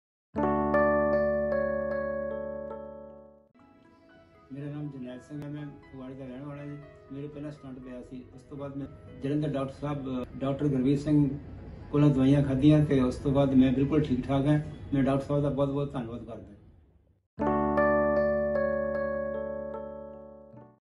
Dr. Gurbeer Singh Gill | Heart Blockage Treatment without Stent | Oxford Hospital Jalandhar
Happy patient sharing his experience of heart blockage treatment without stent.
Watch the video to listen about his experience